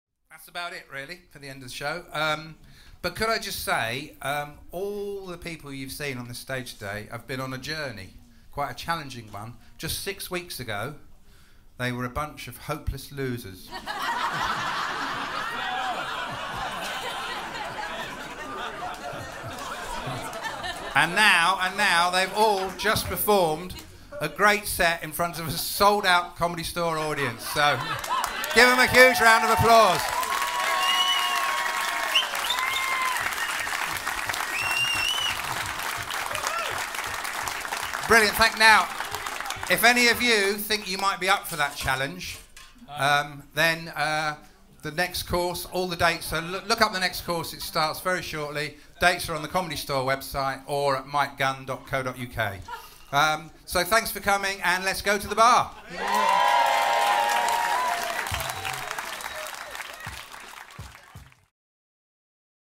Mike Gunn's Beginners' Comedy Course
Course tutor Mike Gunn signs off his student's showcase with a few words.
If you'd like to take the plunge and sign up, head over